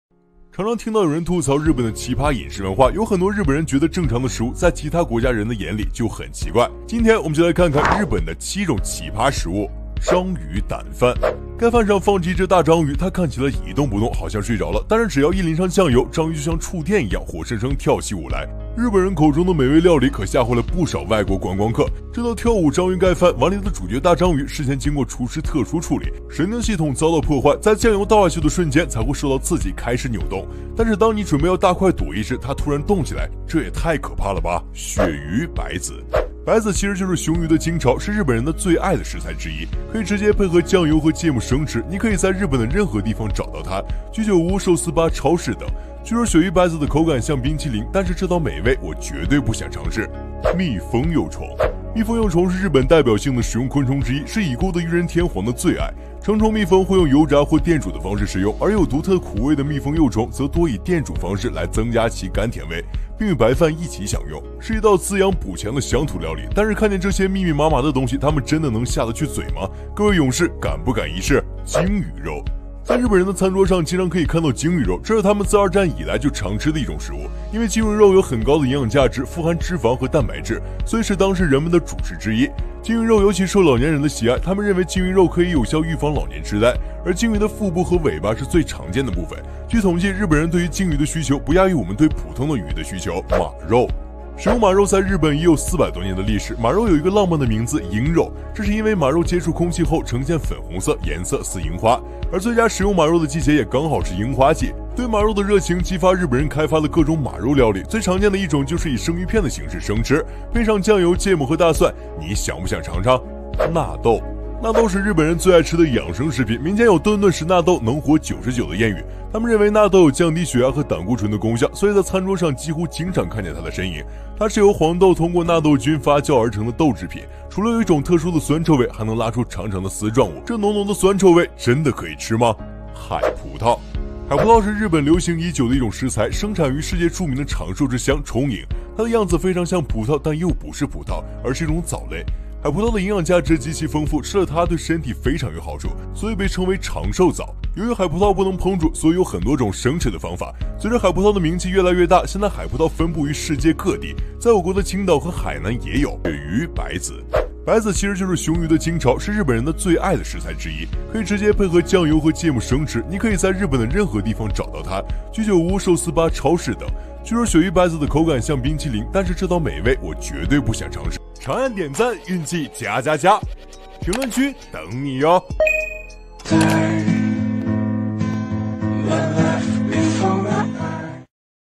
日本的7种奇葩美食，章鱼丼饭生龙活虎，吃前先给你跳一段舞？
『趣闻』看一看赞-为用户提供感兴趣、
有价值的生活健康文章内容及趣剧情搞笑视频。
“让您的阅读观看更有价值”